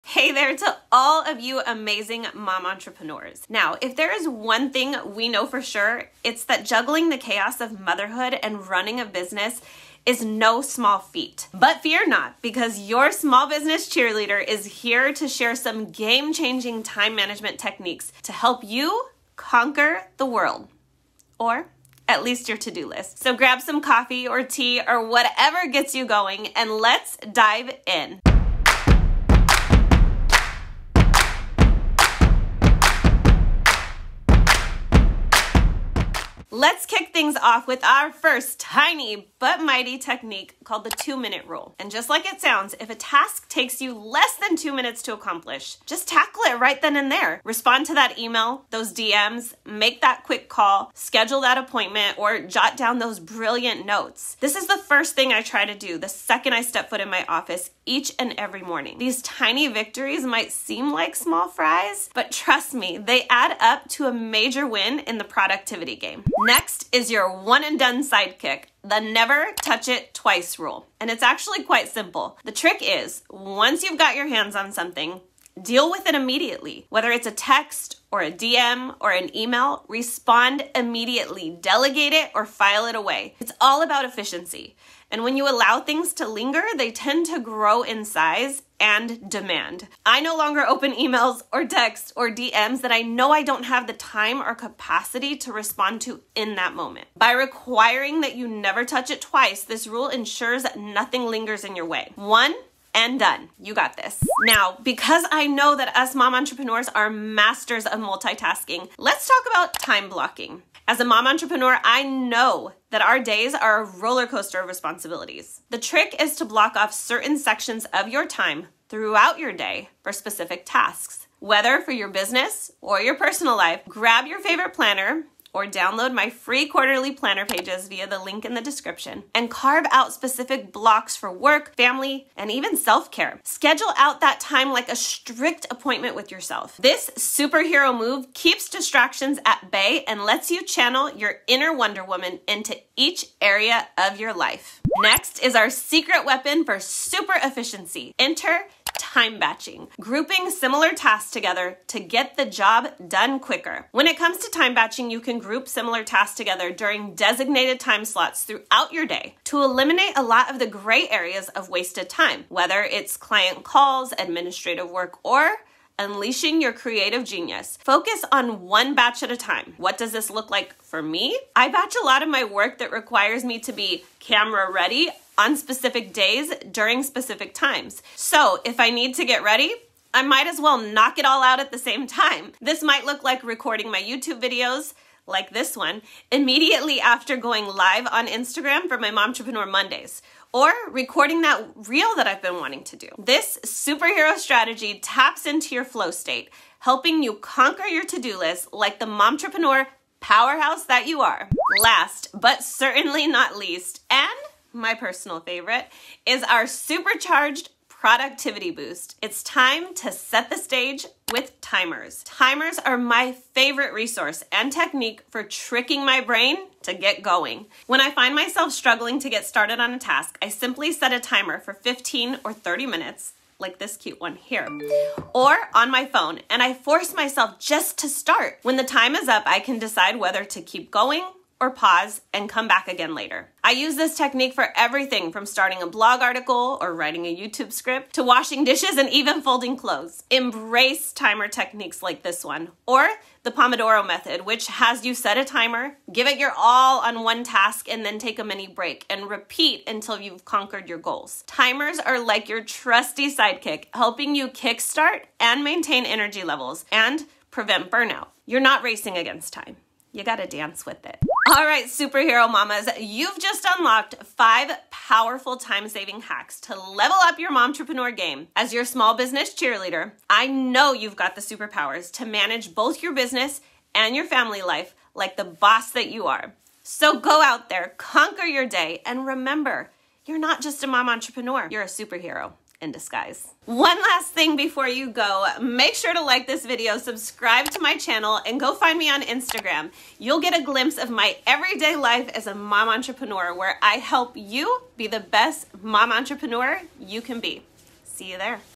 5 Super Hero Time Management Tips for Mom Entrepreneurs
If you're a mom entrepreneur trying to juggle it all but never feel like there's enough time... LISTEN UP!!!

I'm sharing 5 super hero time management tips for mom entrepreneurs to help you optimize your time and get it done!

WANT MY FREE QUARTERLY PLANNER PAGES TO HELP YOU KEEP IT ALL TOGETHER??